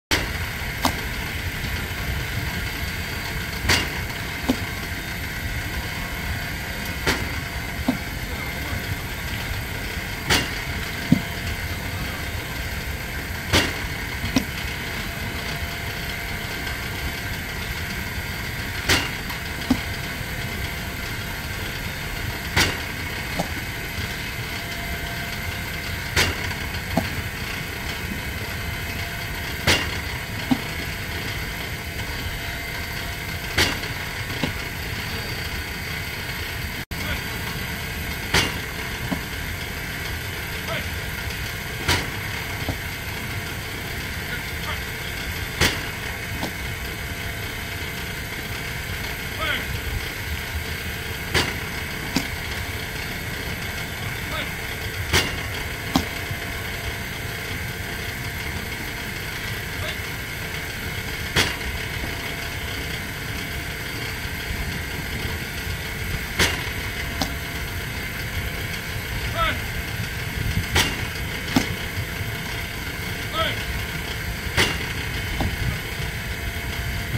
고교야구 유망주 포수들 블로킹 프랙티스 영상 x 덕수고등학교 야구부
고교야구 동계훈련 현장체크 x 더 베이스볼 팩토리